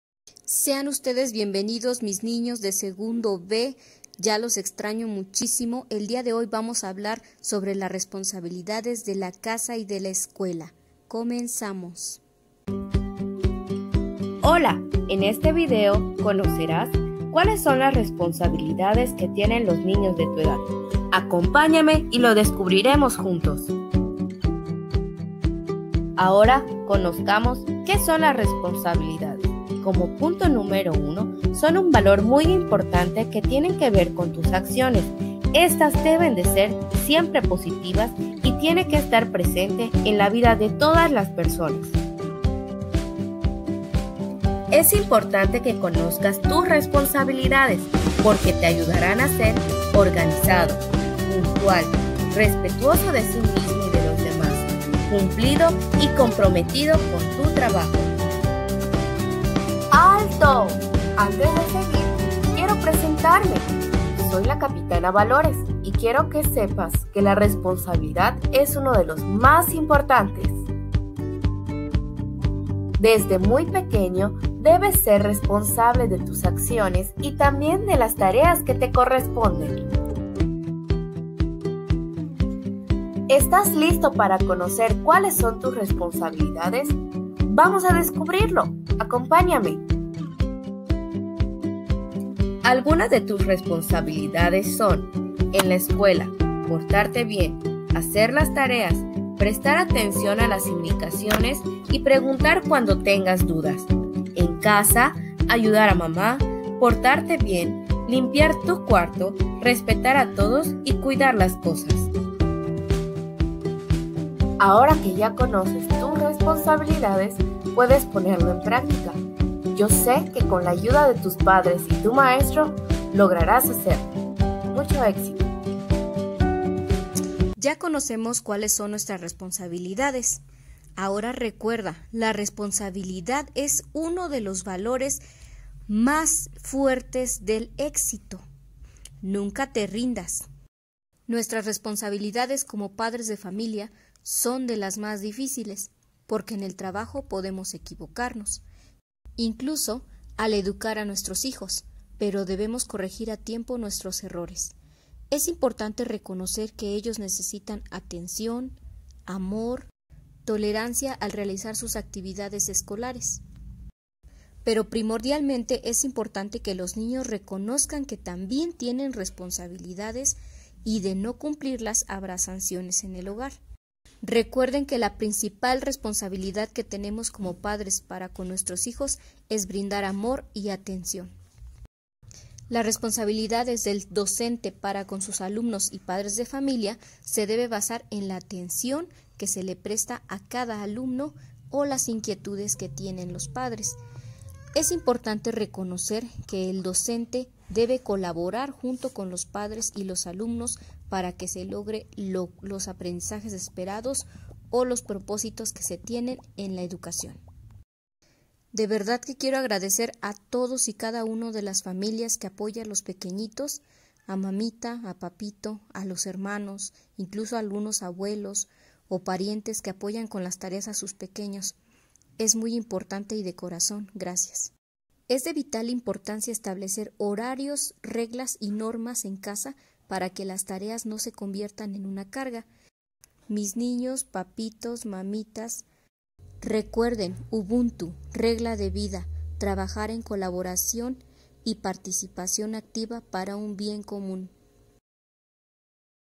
Las responsabilidades del alumno, docente y padres de familia en la educación.
Video dirigido a alumnos de primaria con la finalidad que conozcan las responsabilidades que tienen en su educación.